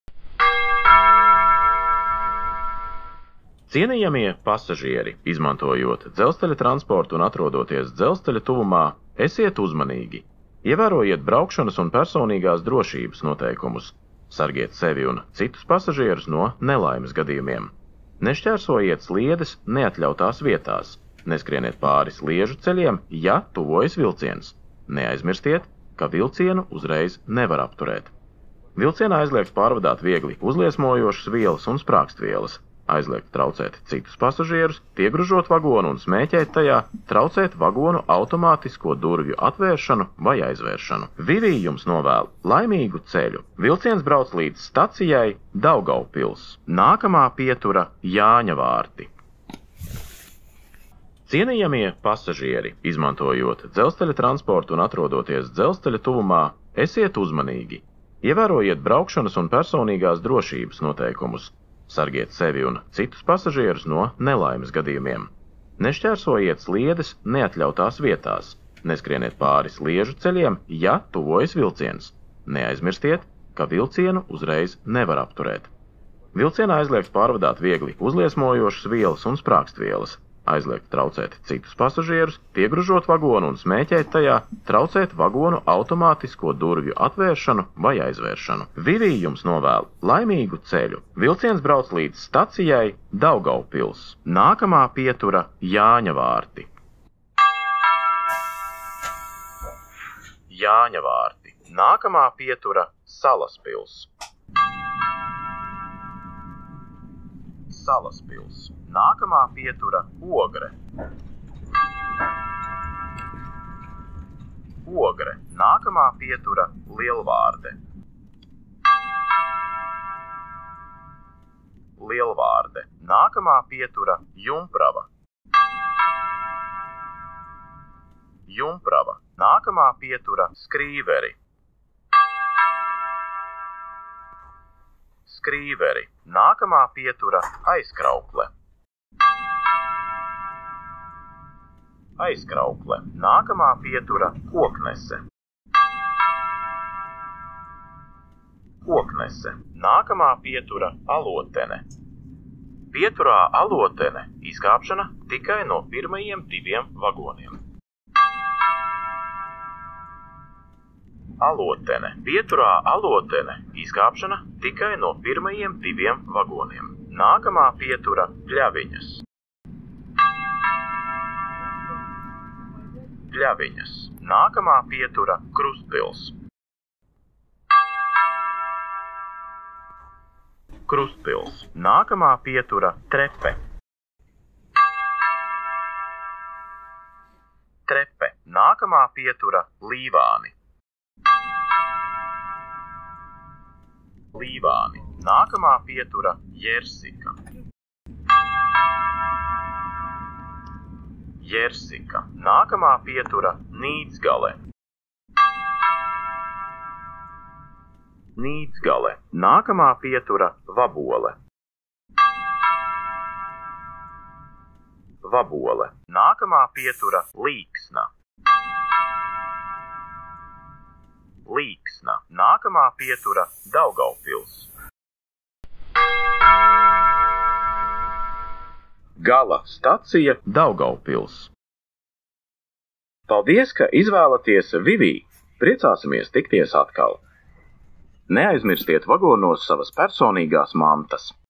Информатор дизель поезда Рига-Даугавпилс (Новый)
Новый информатор дизель поезда Рига-Даугавпилс
Таймкоды:
00:00 - Рига-Пасс.
01:38 - о.п Яняварты
01:45 - Саласпилс
01:51 - Огре
01:58 - Лиелварде
02:06 - о.п Юмправа
02:12 - Скривери
02:19 - Айзкраукле
02:25 - Кокнесе
02:38 - о.п Алотене
02:50 - Плявиняс
02:57 - Крустпилс
03:04 - Трепе
03:11 - Ливаны
03:18 - Йерсика
03:25 - Ницгале
03:32 - Ваболе
03:38 - Ликсна
03:46 - Даугавпилс-пасс.